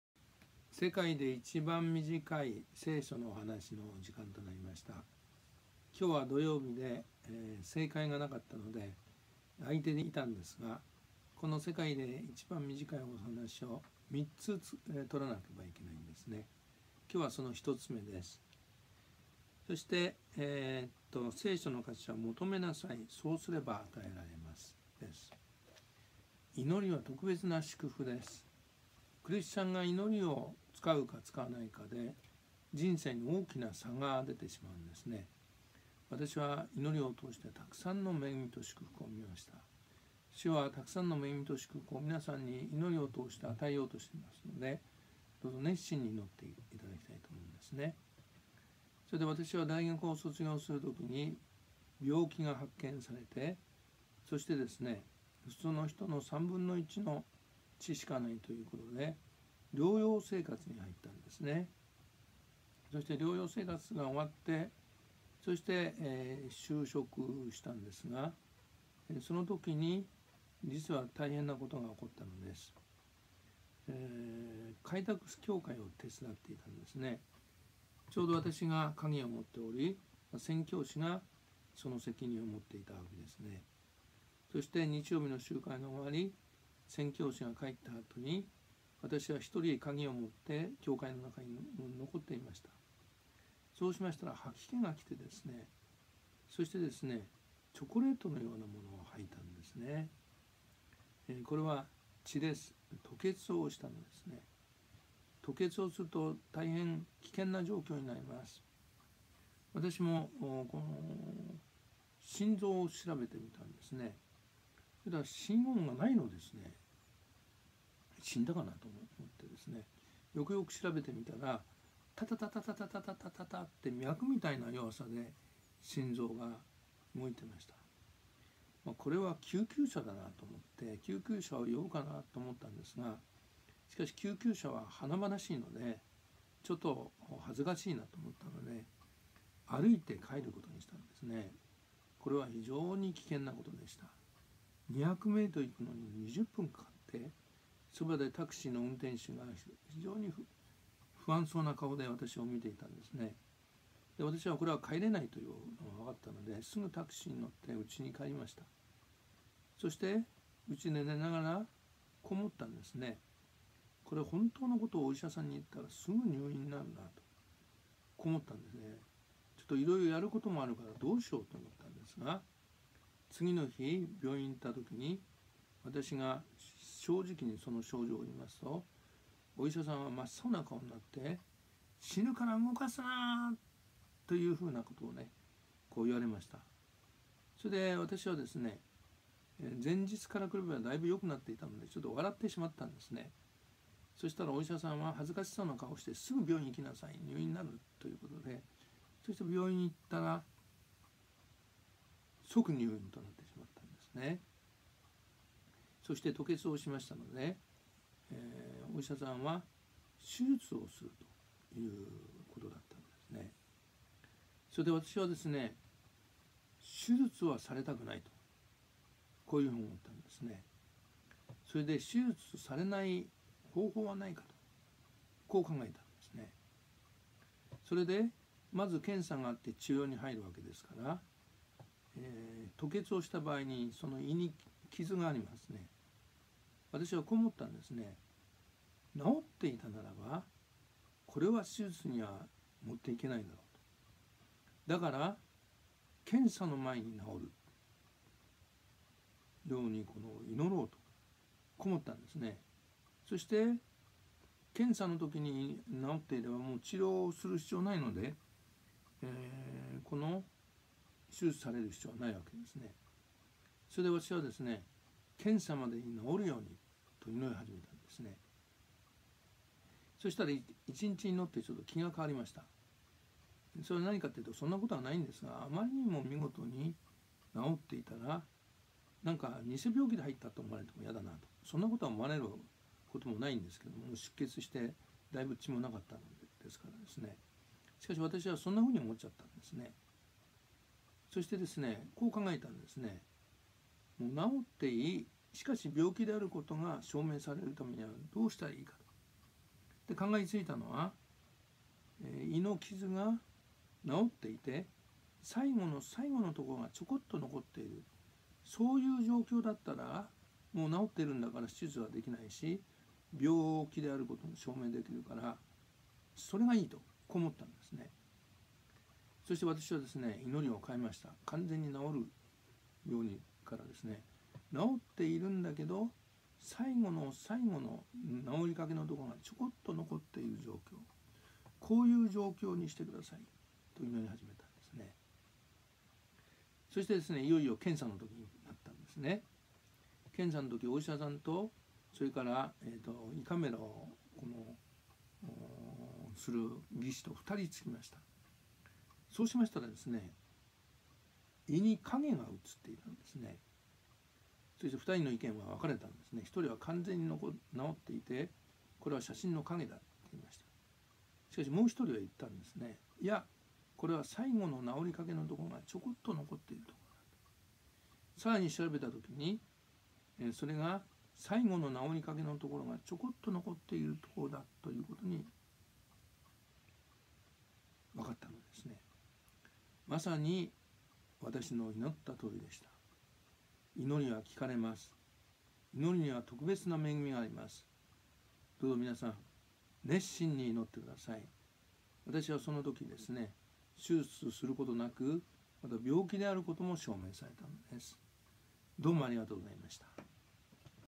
世界で一番短い聖書のお話 NO.341
求めなさい。そうすれば与えられます。
マタイの福音書　7章7節